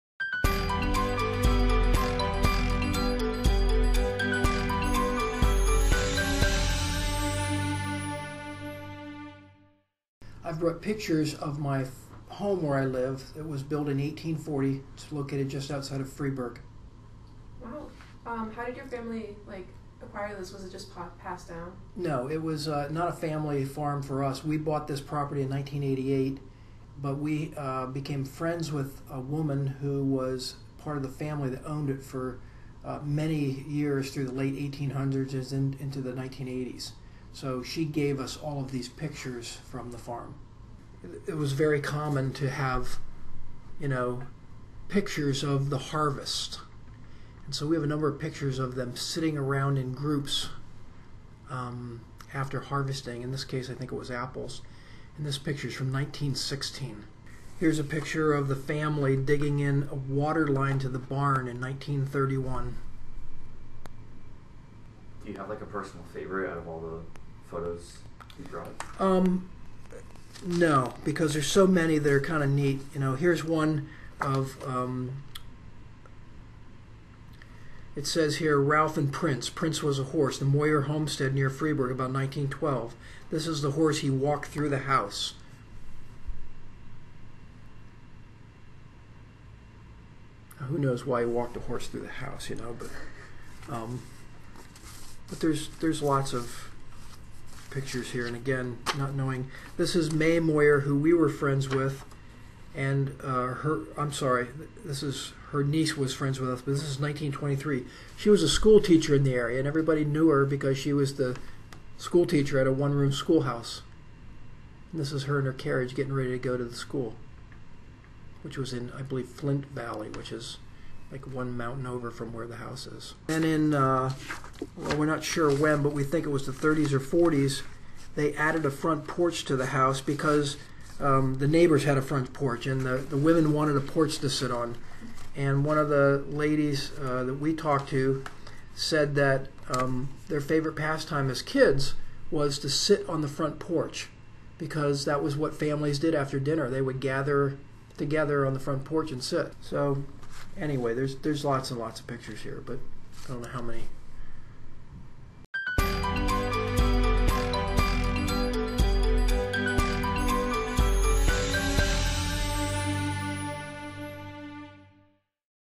Mike Flock History Harvest Interview
Interview with Mike Flock conducted during the Snyder County History Harvest, October 22, 2016